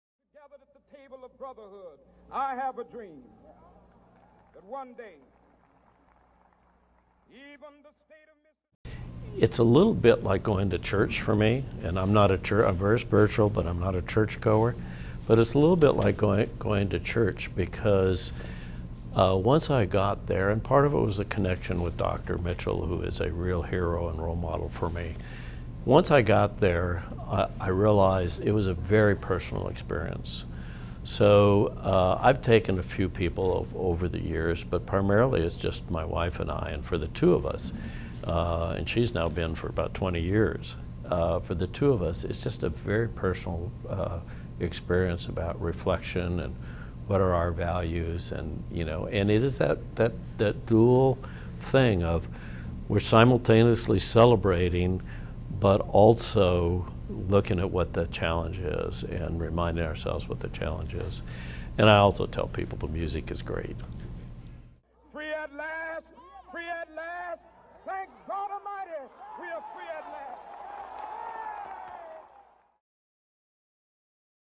Seattle Colleges' Dr. Martin Luther King, Jr. Celebration 2015. Frank Blethen on the Celebration.
Seattle Colleges' Dr. Martin Luther King, Jr. Celebration 2015. The Seattle Times' Frank Blethen discusses the Celebration.
We hope you can attend. The celebration takes place Friday, January 16, 2015, Noon to 1:30 p.m., Mount Zion Baptist Church, 1634 19th Avenue, (corner of 19th Avenue and East Madison Street) in Seattle.